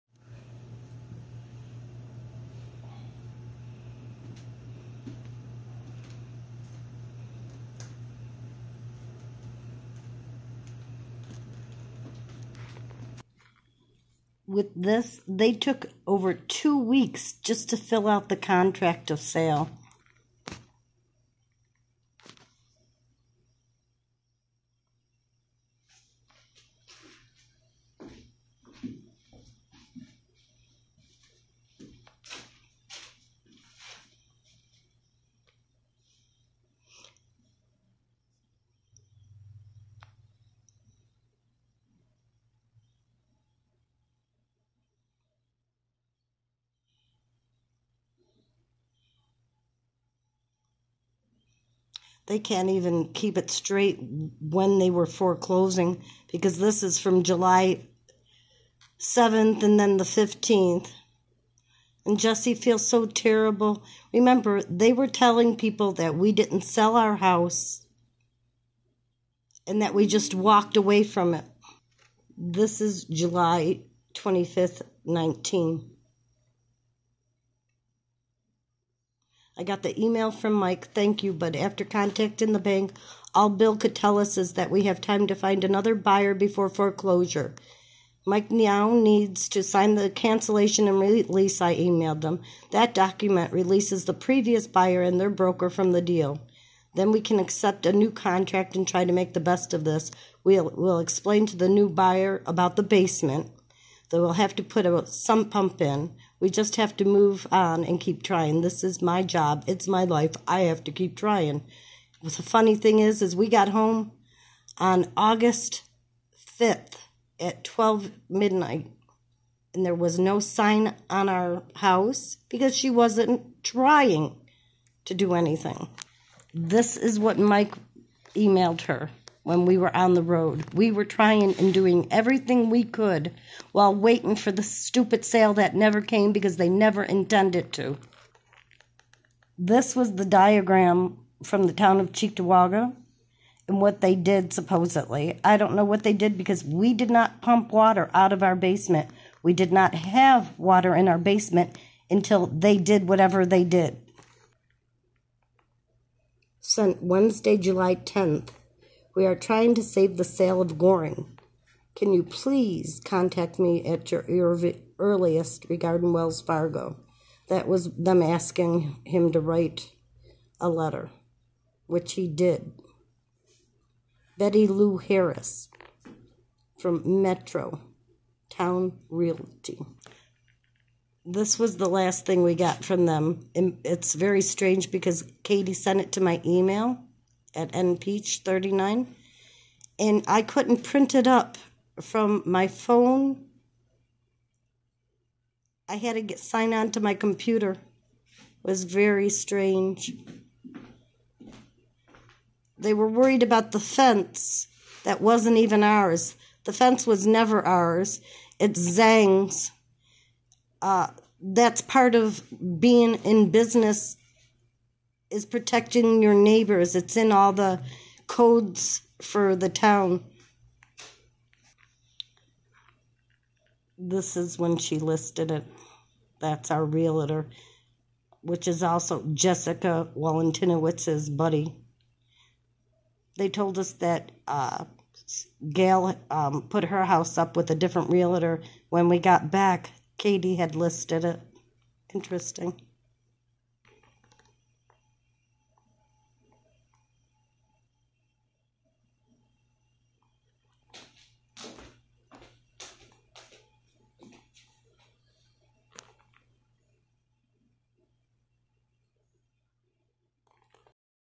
Documents from the "sale" of our house as well as the “fake plumbing inspection” papers
Niagara county does not do work in people’s homes- but Cheektowaga can flood peoples home and make it their problem?! Here’s an example of what we’re talking about Please ask SenatorSchumer and perhaps CongressmanHiggins If fraud, harassment, vandalism, sleep deprivation, intimidation acceptable in Western New York? Now understand; first the domestic terrorism, (to get us out of the house and separate us from our belongings) fraud on the sale, accusations of abandoning our home, accusations of self harm and or violence We're not the ones that have been violent!

the sale of our house was supposed to be a quickie, two months..   he wasn't asking for an inspection, he was pre-approved for.. our realtor Katie waited for us to leave and then dropped the Town Plumbing inspection on us. She was here on March 29 and never said a word to us. WHY would anyone wait 6 months to do their walk through that was ready at least 3 months?! for a#Fraud that's why!